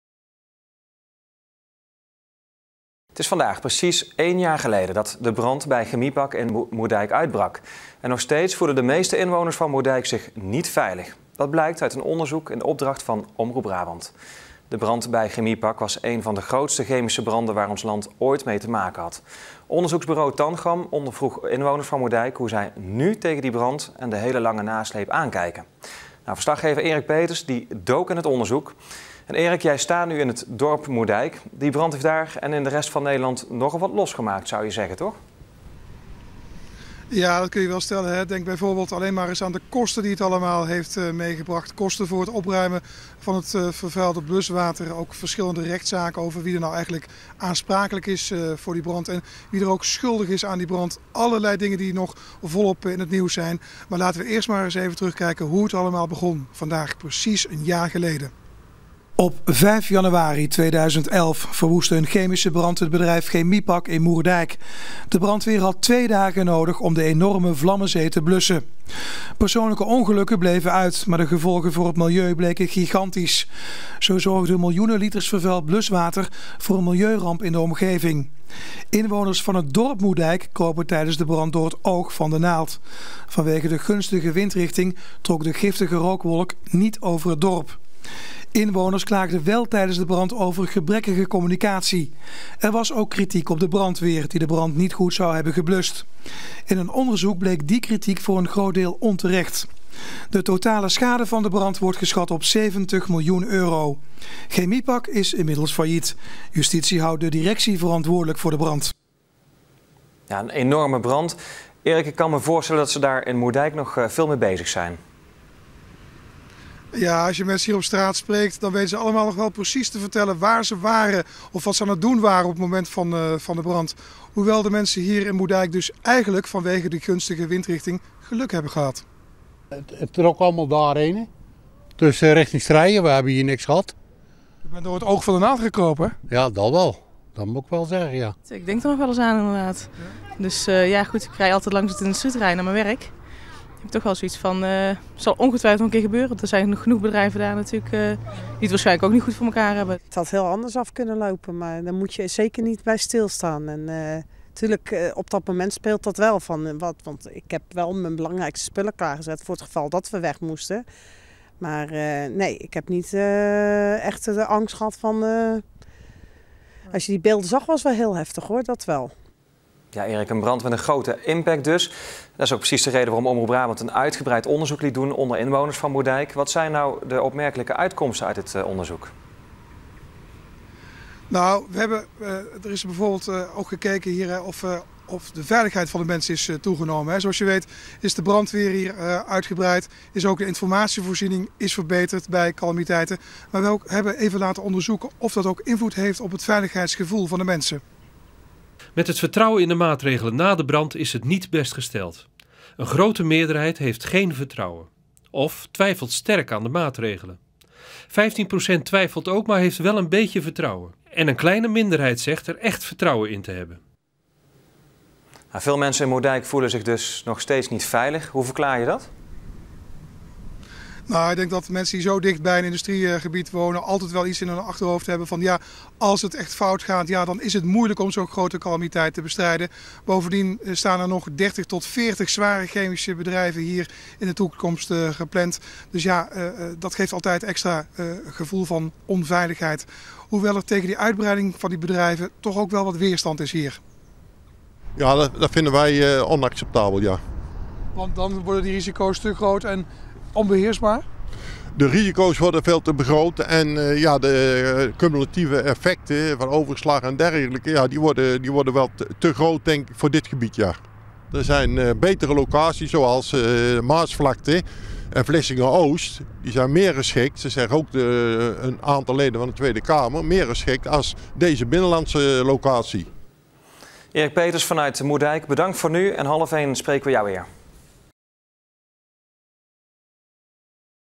Verslaggever Erik Peeters over de veiligheid na de brand bij Chemie-Pack
- Moerdijk - Zeventig procent van de inwoners van Moerdijk denkt dat het voor omwonenden na de brand bij Chemie-Pack in Moerdijk niet veiliger is geworden, ondanks maatregelen die genomen zijn. Dat blijkt uit een onderzoek door bureau Tangram, in opdracht van Omroep Brabant. De brand bij Chemie-Pack ontstond exact een jaar geleden, op 5 januari 2011.